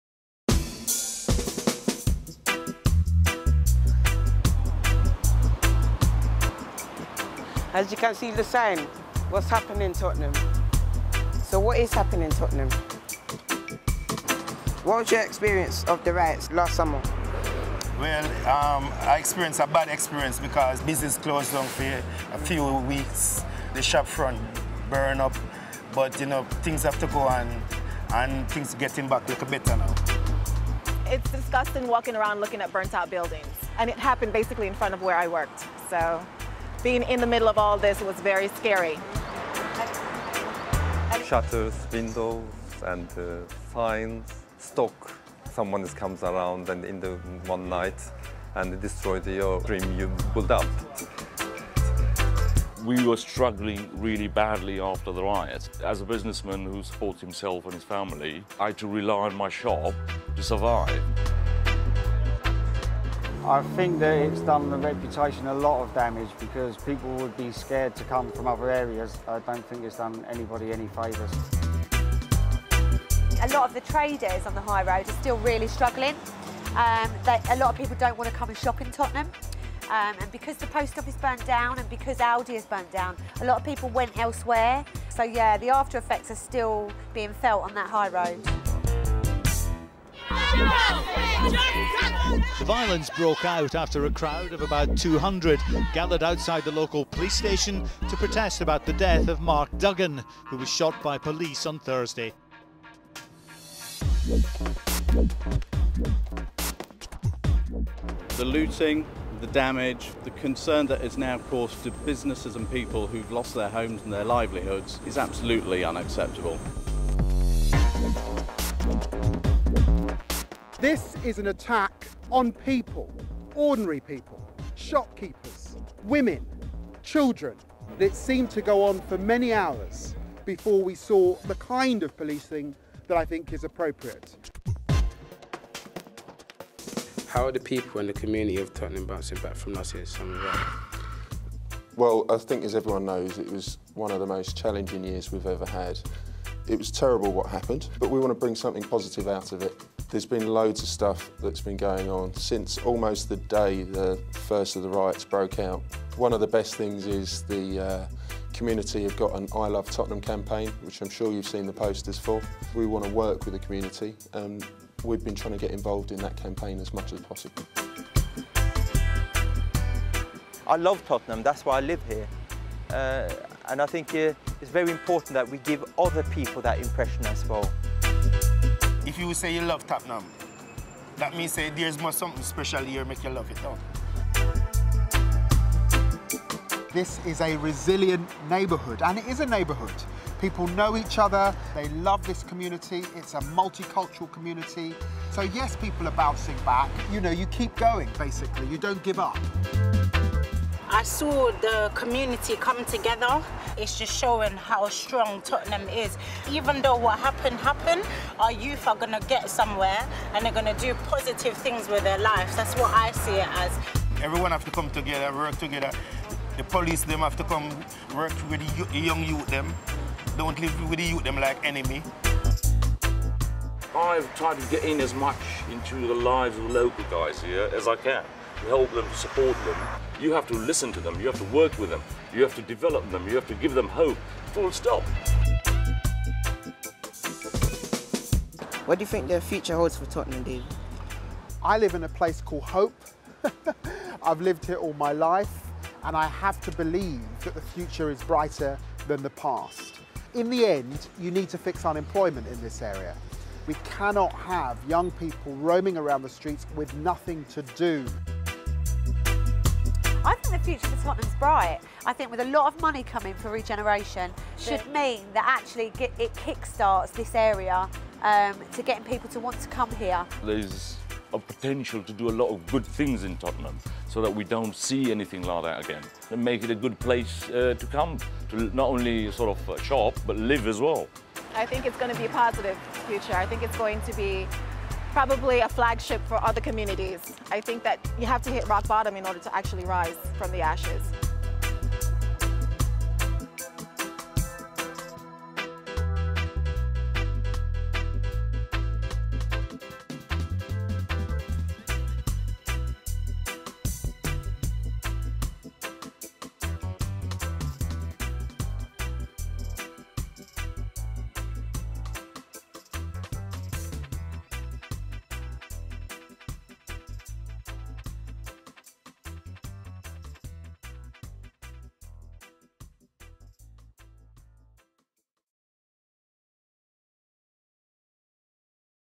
What's Happening Tottenham? A Boxing Academy student film - August 2012
A report on the state of Tottenham one year after the riots by local young people. A Media Trust - Boxing Academy project.